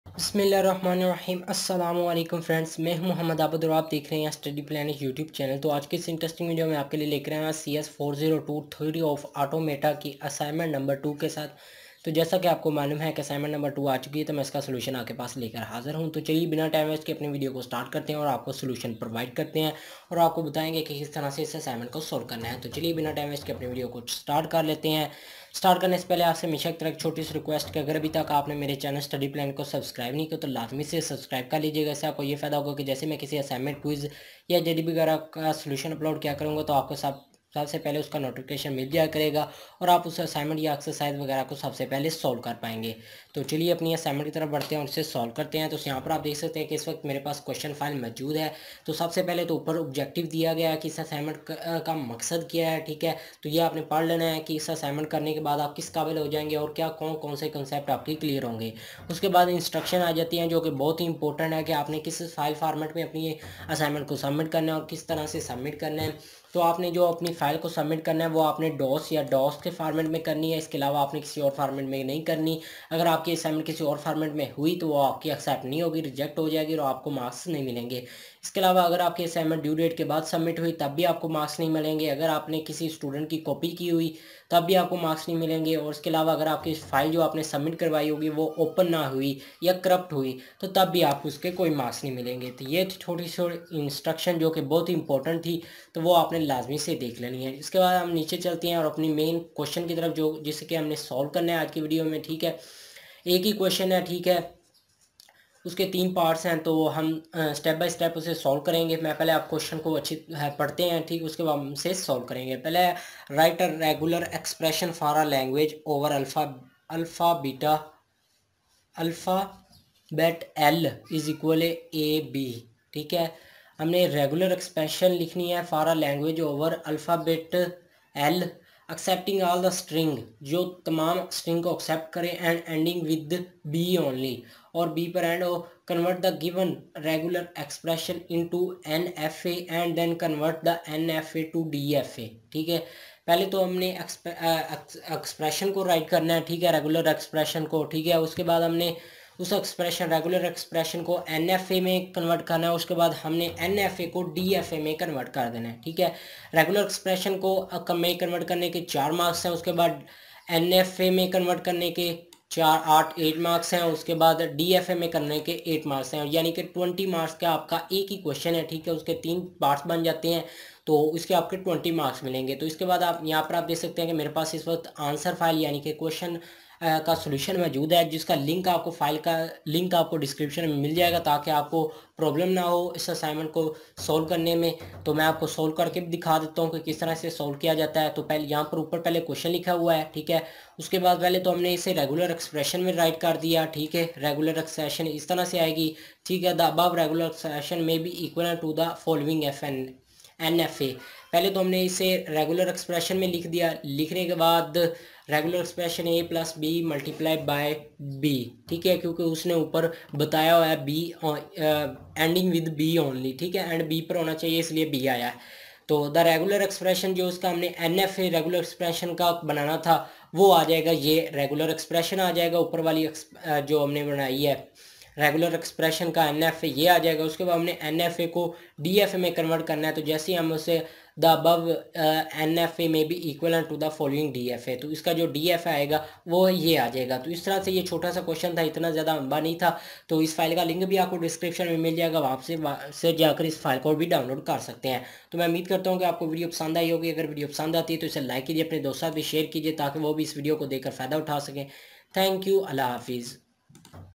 CS402 Assignment No 2 Solution Spring 2020 | Study Planet
Hi! Guys
Study Planet

In this video i will tell u how to solve CS402 Assignment No 2 Spring 2020 | Study Planet.

How To Copy VU Quiz And Search On Google | VU Quiz Allow Copy | Technical Urdu New